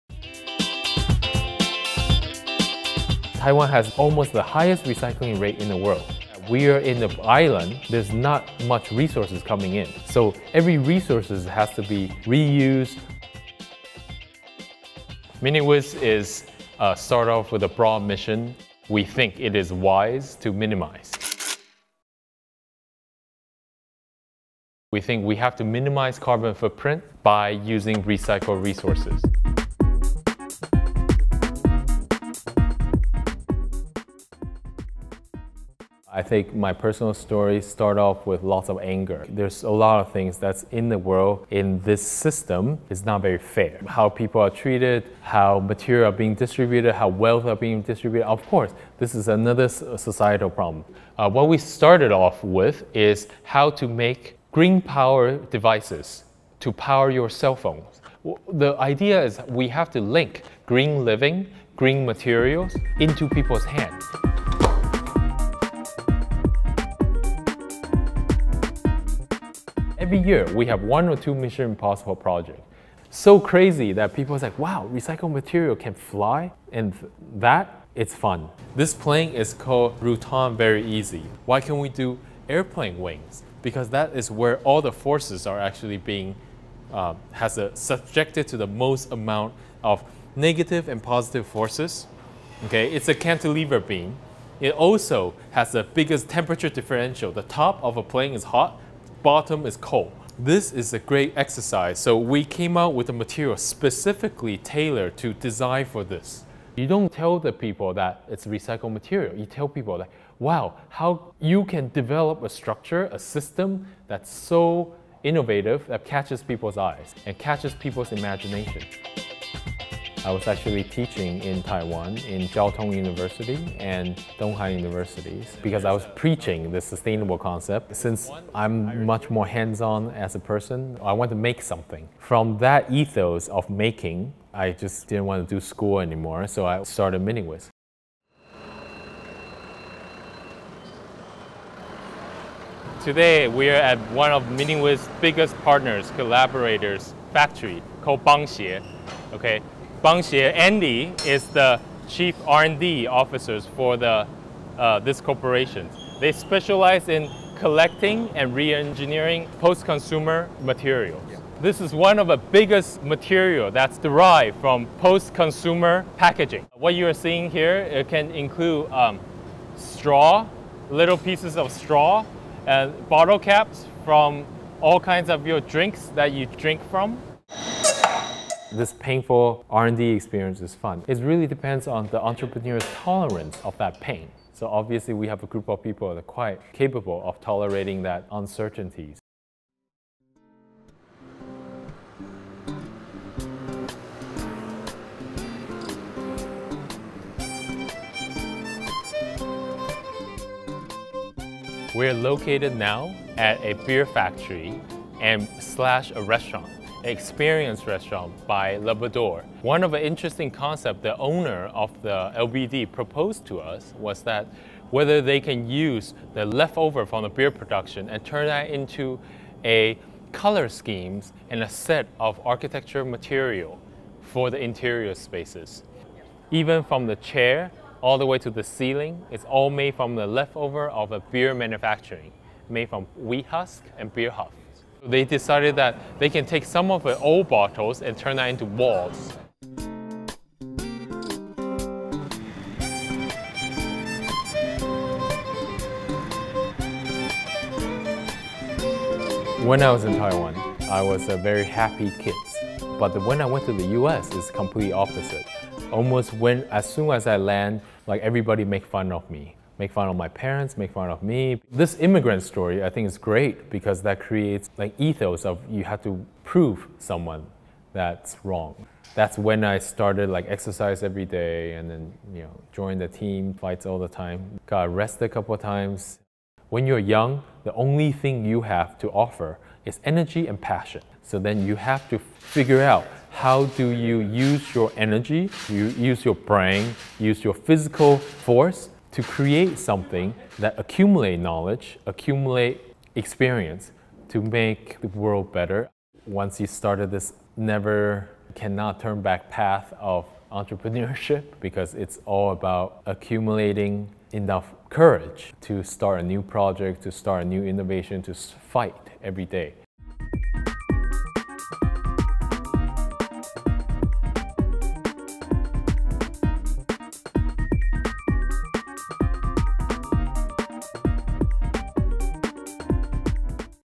How to Build a Business Out of Trash
Arthur Huang, the CEO and co-founder of the Taiwan-based company Miniwiz, turns recycled plastic materials into exhibition spaces, restaurant interiors and airplane wings. Video: Joyu Wang Photo: Diana Jou